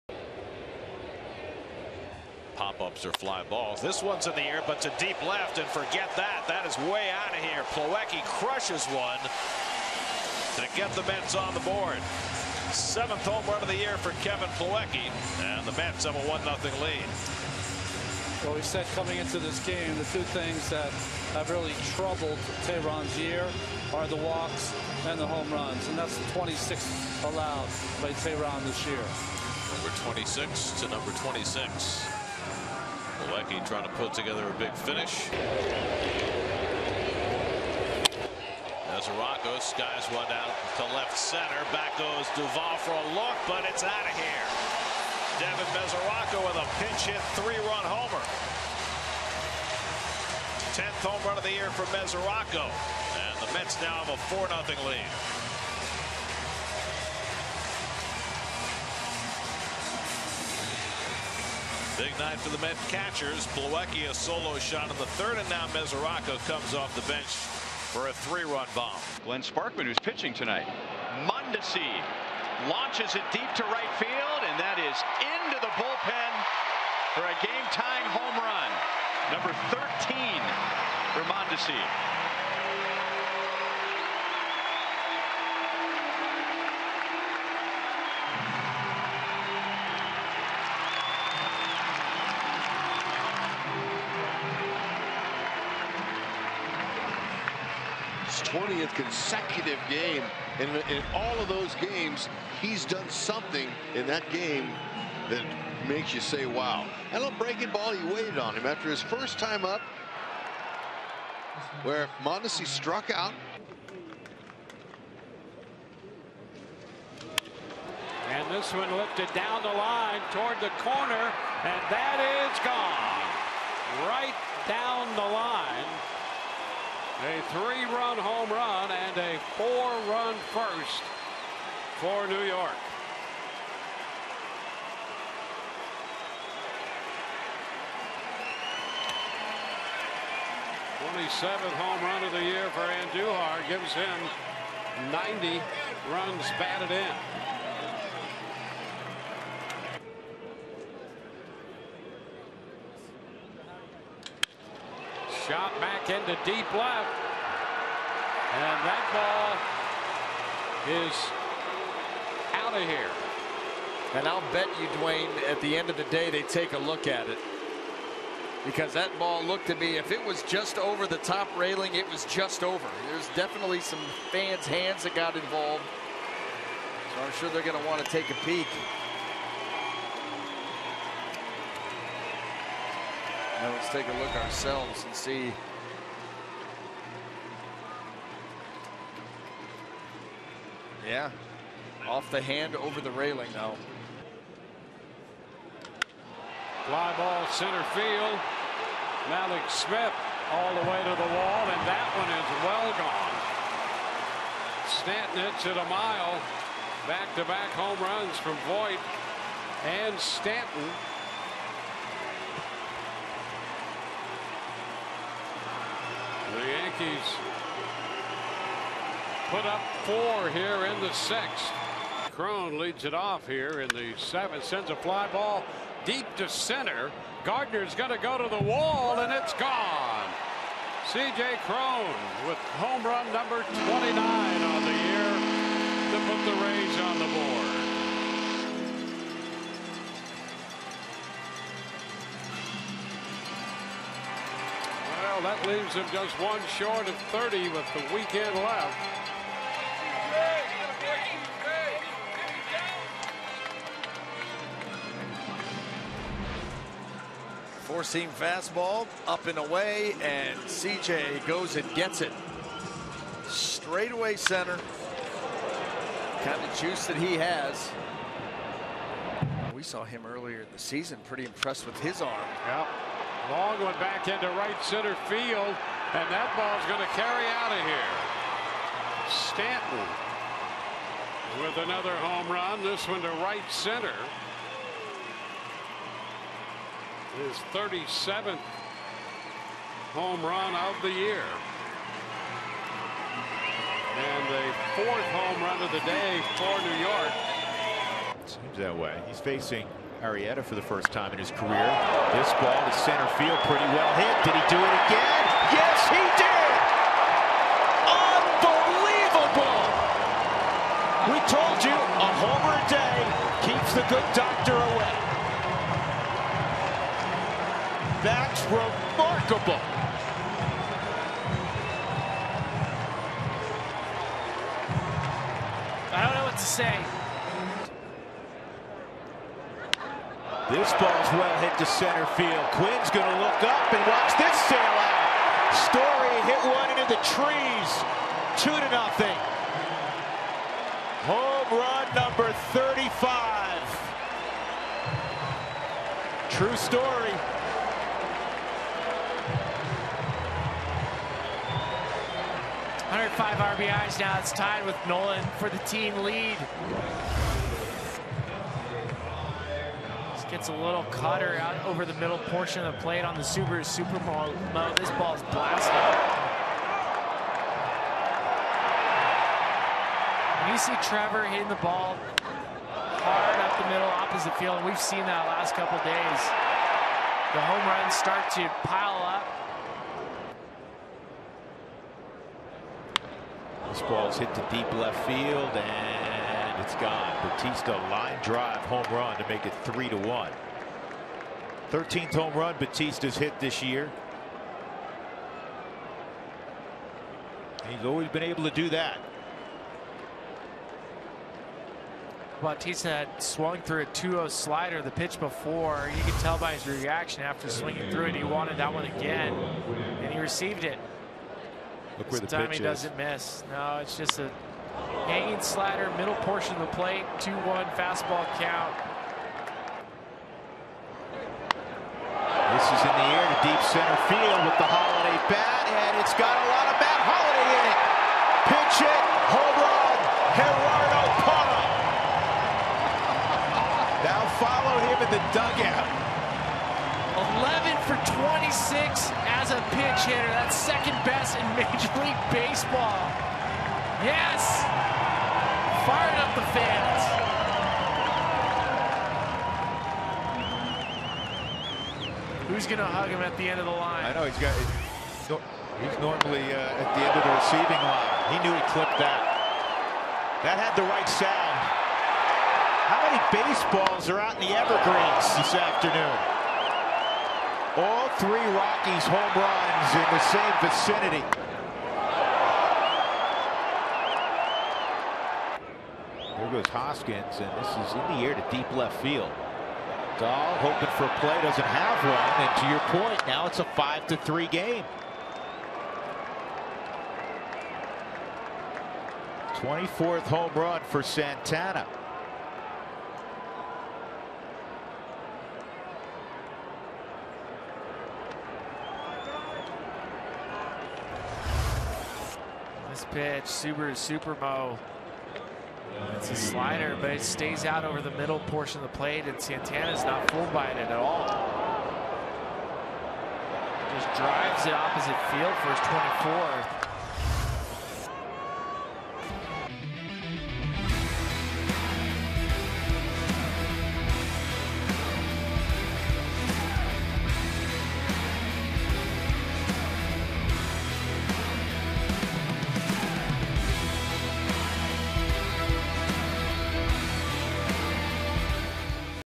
All MLB homers from September 27, 2018
Check out all 13 homers from Thursday, September 27th, including the Rockies’ big blasts and Giancarlo Stanton’s two homers in Tampa